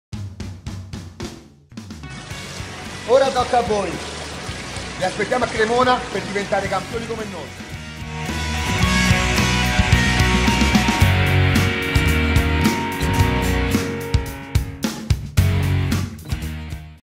Diventa un campione come noi!! Trofeo kinder+sport mini-pallanuoto
I campioni del 7bello Christian e Nicholas Presciutti lanciano la sfida...300 piccoli pallanotisti parteciperanno al Trofeo Kinder+Sport Tutti in Piscina (12-13 Aprile Cremona).